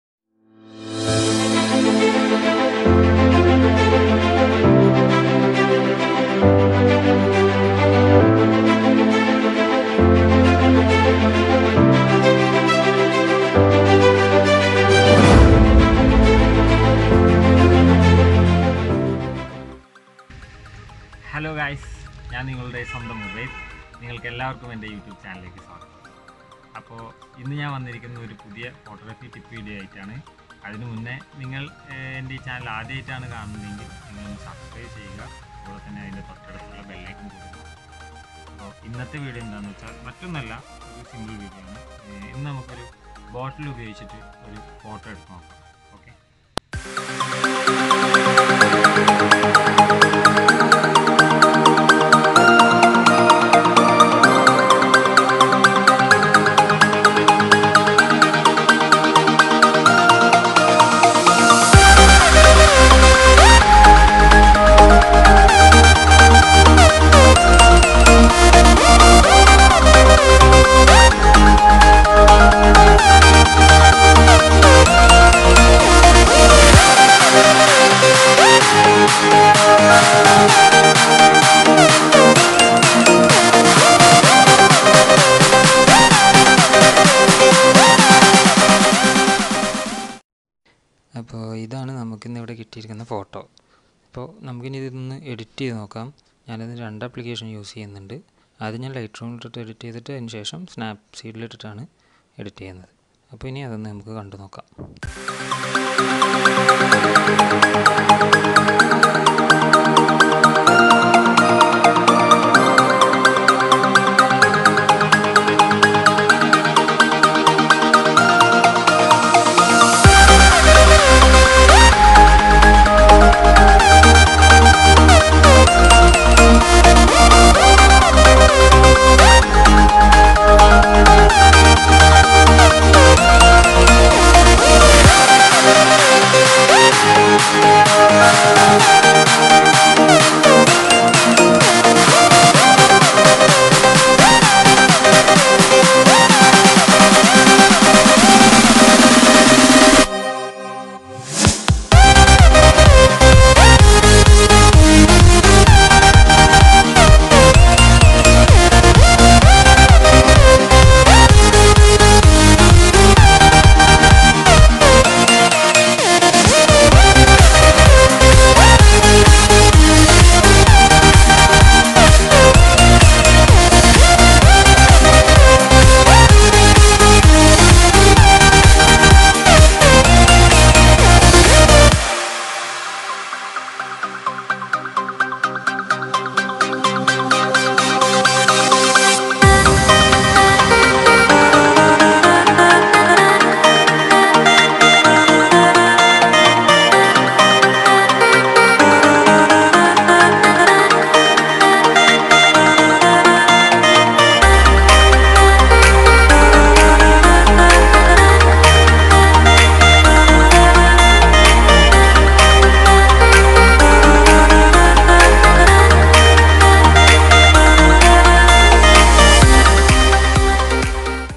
PHOTOGRAPHY TIPS AND TRICKS #3 || photography with a bottle || canon || lightroom || snapseed.
PHOTOGRAPHY TIPS AND TRICKS #3
In this video you can see how a photo is made and the edits done to a photo.

►► PHOTO SETTINGS:

CANON 1300 D || CANON EFS 55-250 MM
APERTURE :  F/4.5
SHUTTER SPEED : 1/1600 SEC
ISO : 200

► This video is related to photography tricks and tips.